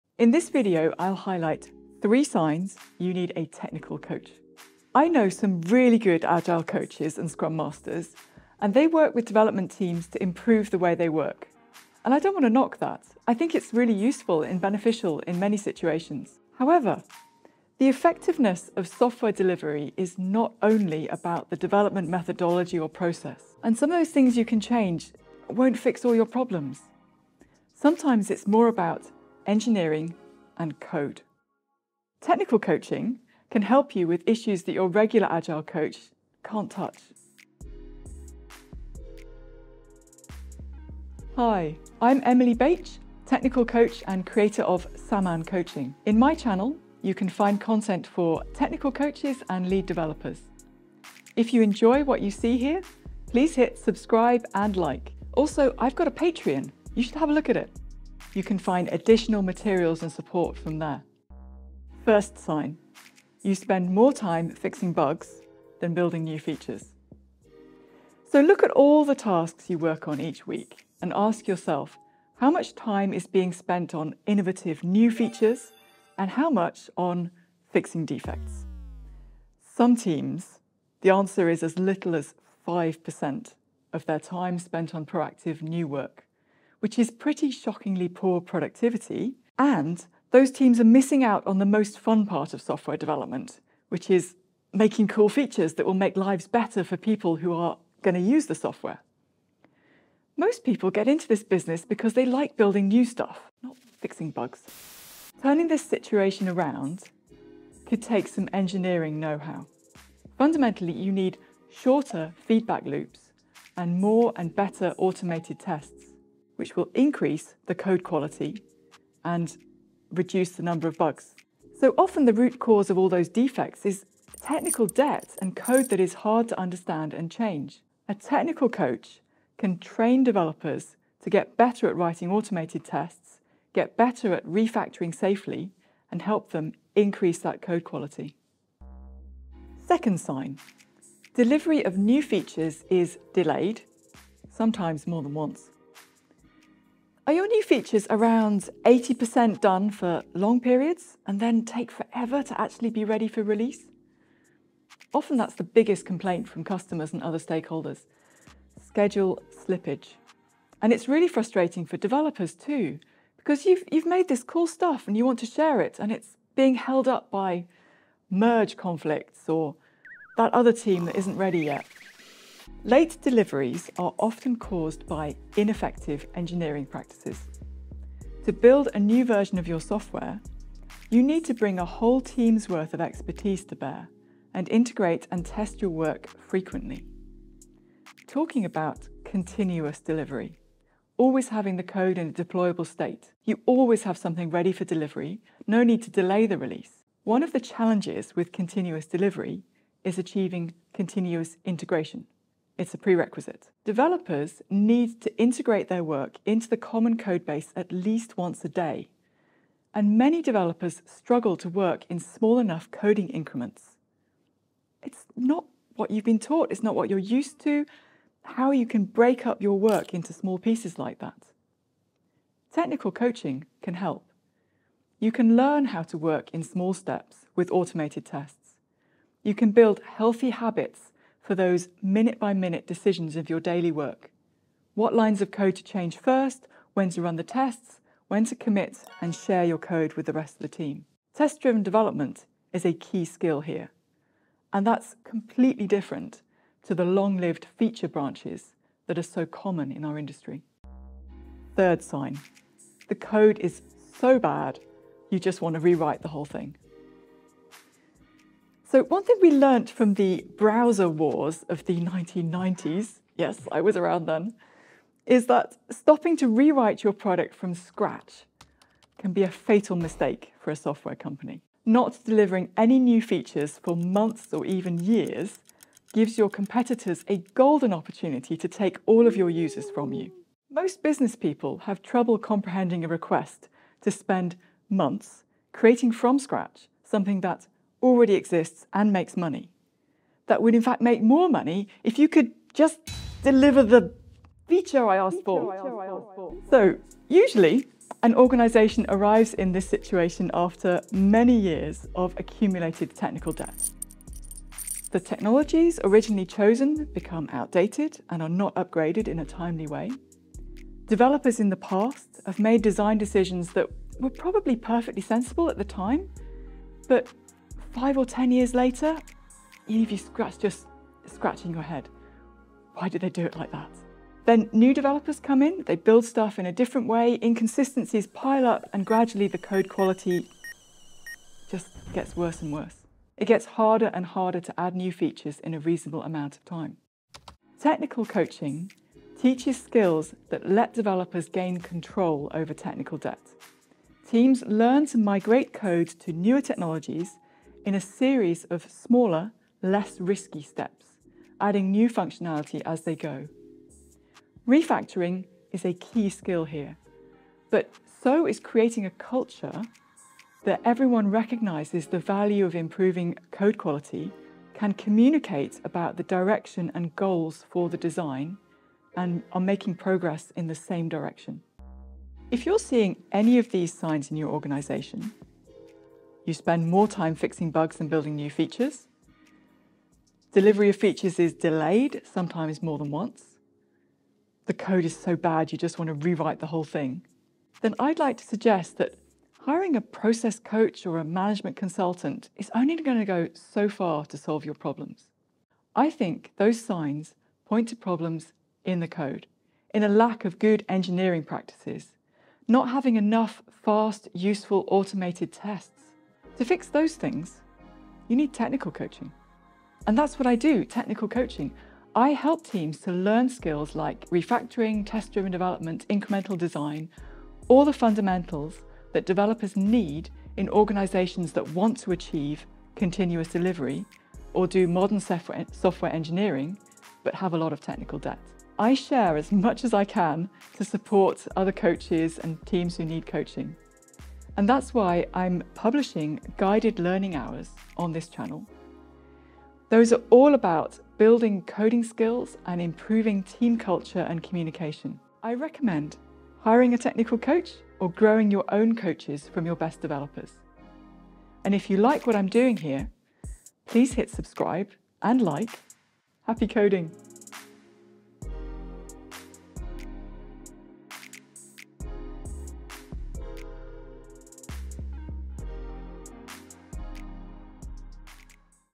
3 SIGNS You NEED a Tech COACH
In software development, sometimes you run into difficulties where an agile or process coach will struggle to make a real difference. In this video, Emily Bache explains three signs you need a Technical coach.

▬ Video chapters ▬▬▬▬▬▬▬▬▬▬
00:00 - Introduction
01:03 - First sign: Bugs
02:19 - Second sign: Scheduled slippage
04:11 - Third sign: Bad code
06:12 - Conclusions
06:46 - Getting a Tech Coach